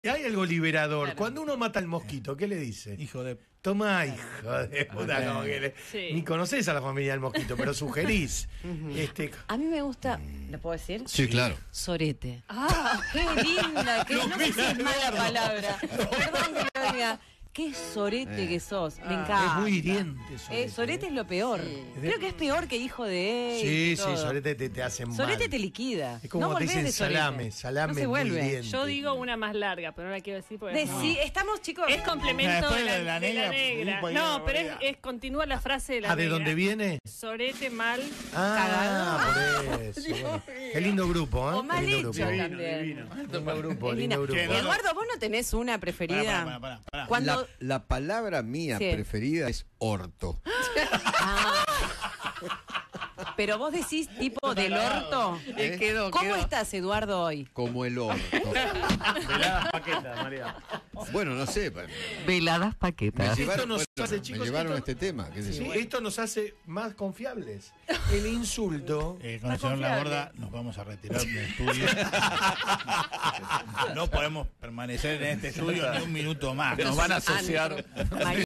Eduardo Feinmann reveló cuál es su insulto preferido y descontroló el aire de Radio Mitre
Durante el pase de la mañana de Radio Mitre, Roberto Moldavsky quiso saber cuáles eran los insultos favoritos de los conductores y Eduardo Feinmann hizo estallar de risa a la mesa.